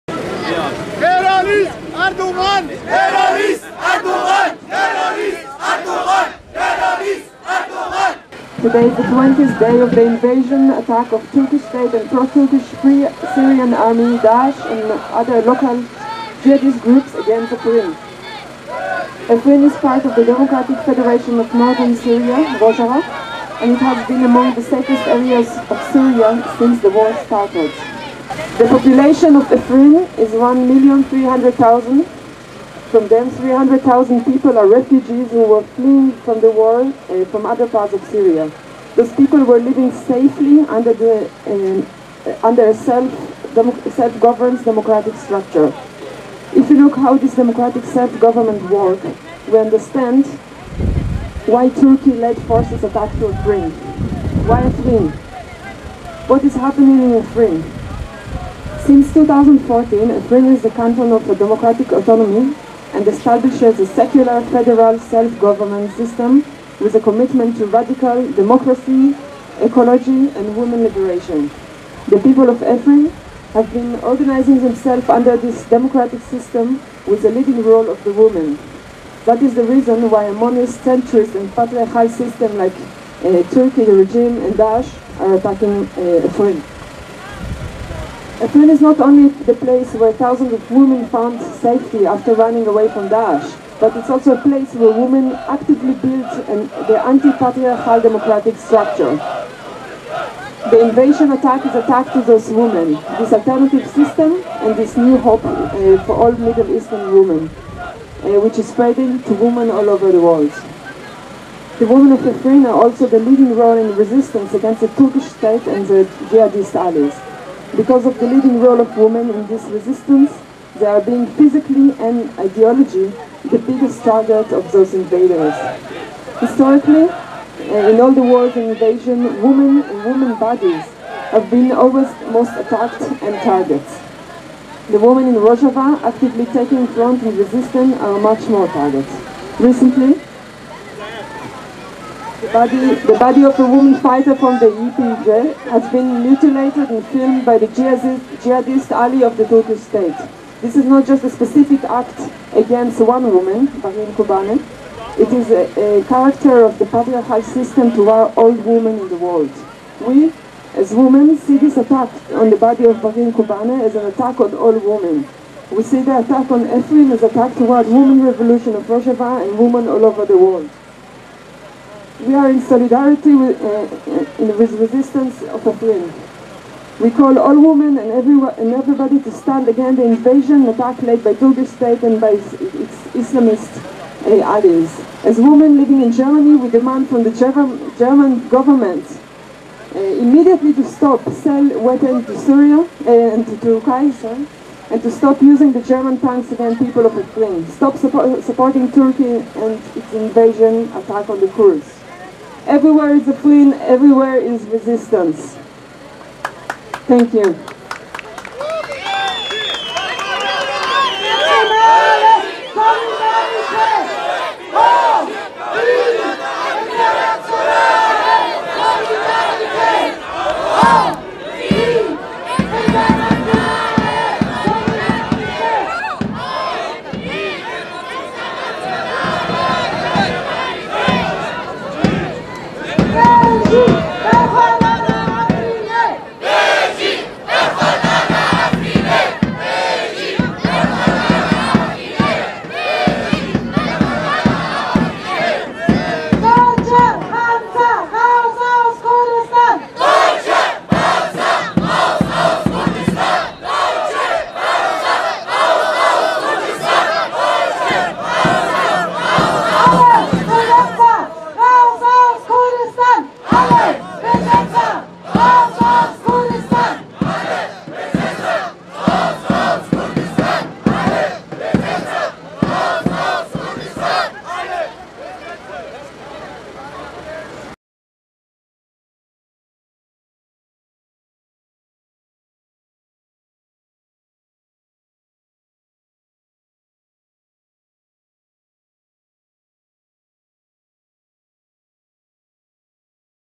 "Stand against the invasion attack led by turkish & its islamist allies!" (Intern. Women Space)
Berlin, Alexanderplatz, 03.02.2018. Kundgebung gegen den Angriff auf Afrin, Rojava.
Motto: No pasaran! - Frauen kämpfen gegen den Faschismus
"Since 2014 Afrin establishes a secular, federal and selfgovernment system with a commitment to radical democracy, ecology and women libertion. We call all women and everybody to stand against the invasion attack led by turkish and its islamist allies!" (International Women Space) International Women Space
Afrin, Rojava saldırısına karşı miting yapın.
Slogan: Pasaran yok! - Kadınlar faşizme karşı savaşıyor
"Afrin, 2014 yılından bu yana, radikal demokrasi, ekoloji ve kadınların özgürlükçülüğüne bağlı bir laik, federal ve kendi kendini yöneten bir sistem kuruyor, tüm kadınları ve herkesi türk ve islamcı müttefikleri tarafından yönetilen işgal saldırısına karşı durmaya çağırıyoruz!" (International Women Space) International Women Space